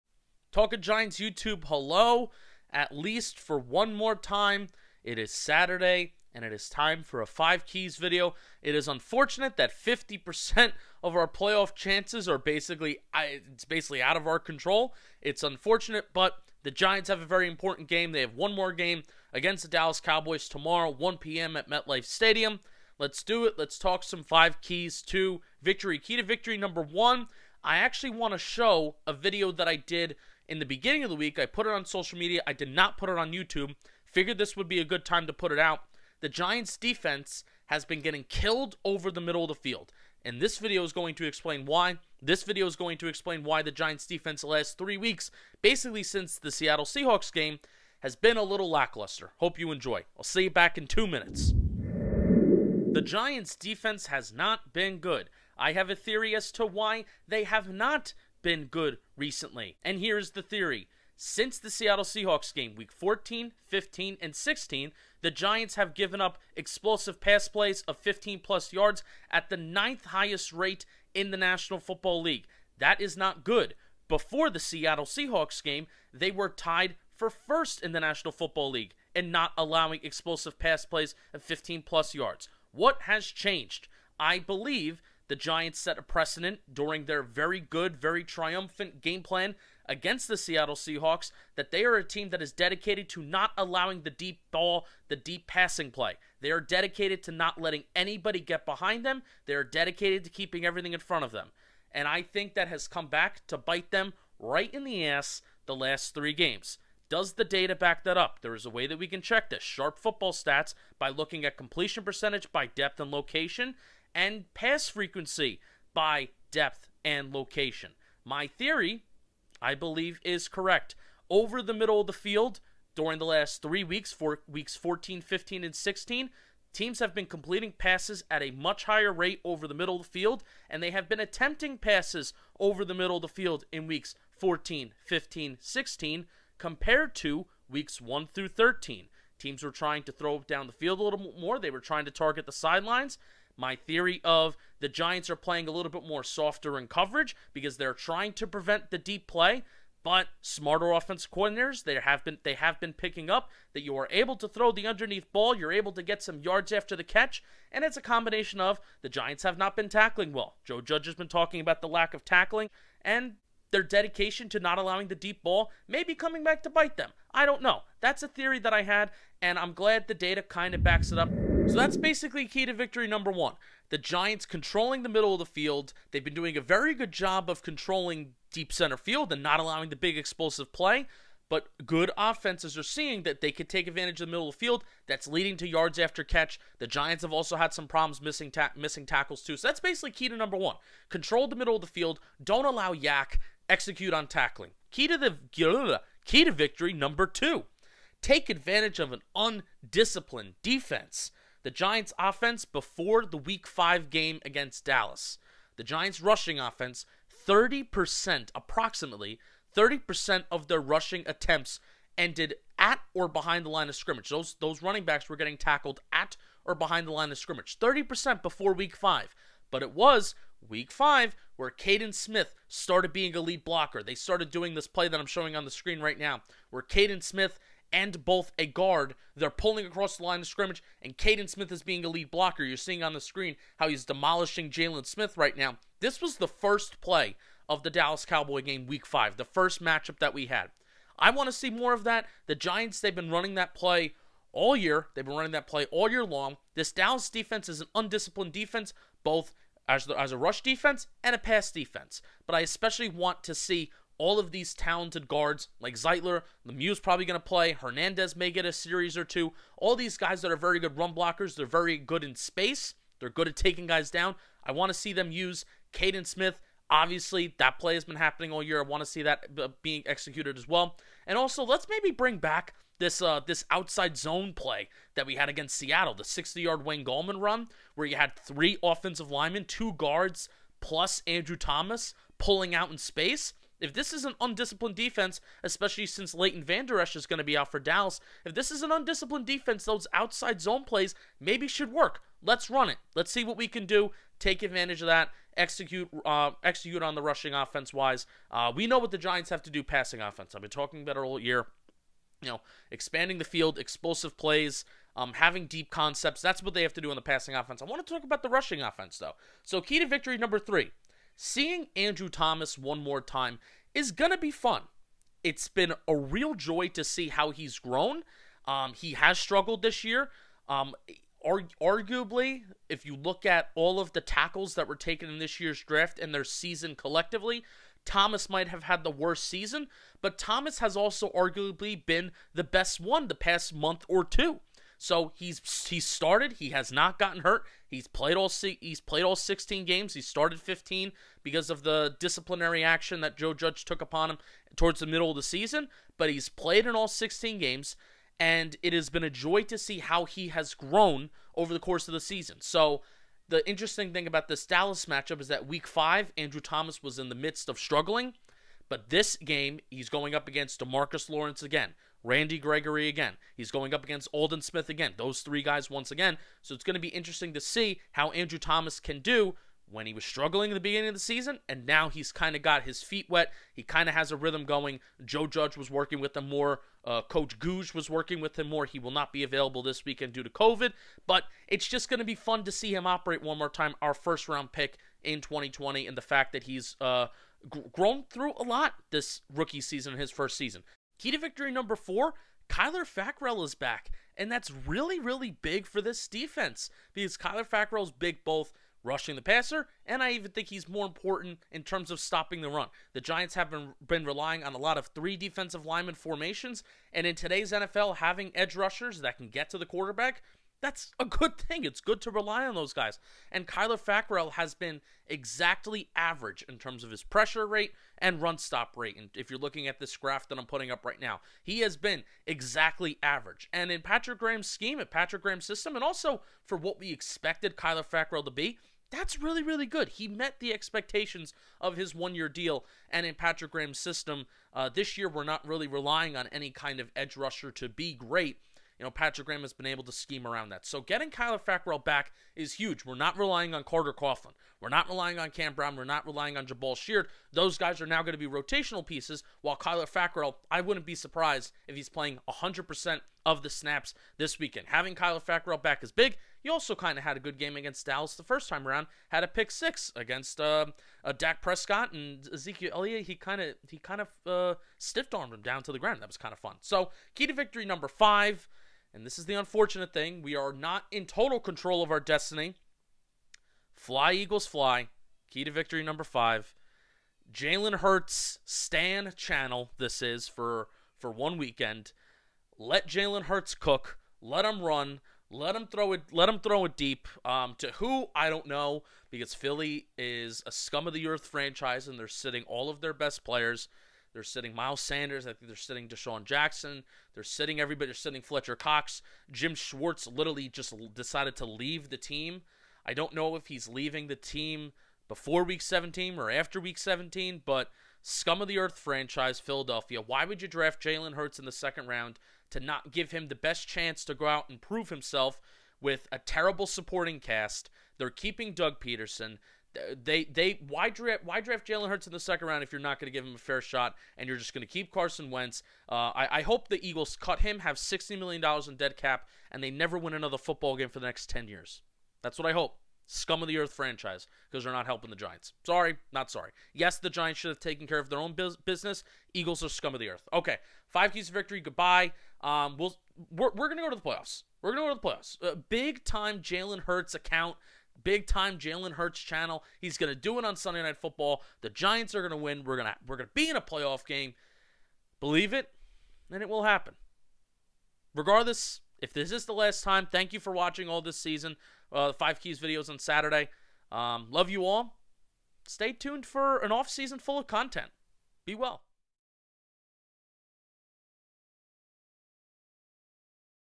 5 Keys to Victory vs. Dallas & What’s Wrong w/ the Defense?
For at least one more time, Justin breaks down what the five keys to victory are against the Giants week 17 matchup including what’s wrong with the defense, the importance of Kyler Fackrell and how the Giants rushing offense can attack an undisciplined Cowboys defense!